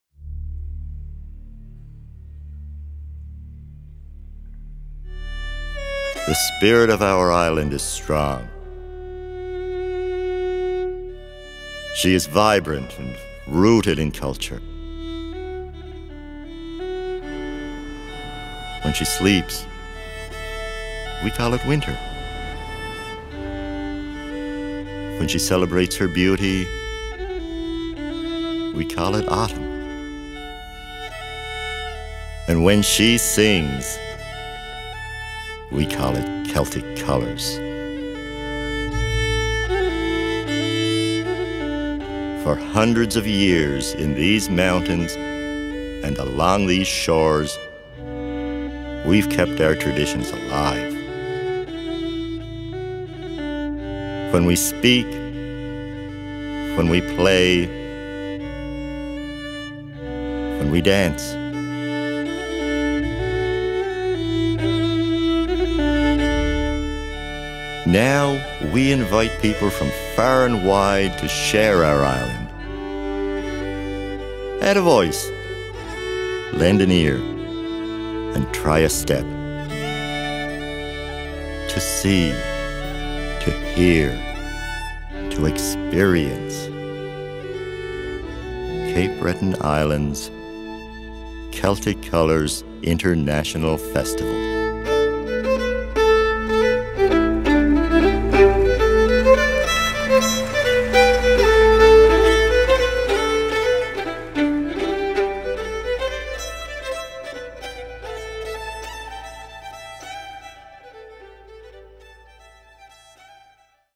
Cape Breton Island's Celtic Colours
The spirit of our Island is strong - she is vibrant and rooted in culture. When she sleeps, we call it winter. When she celebrates her beauty, we call it autumn. When she sings, we call it Celtic Colours.

Join us October 6-14, 2017 for Cape Breton's island-wide celebration!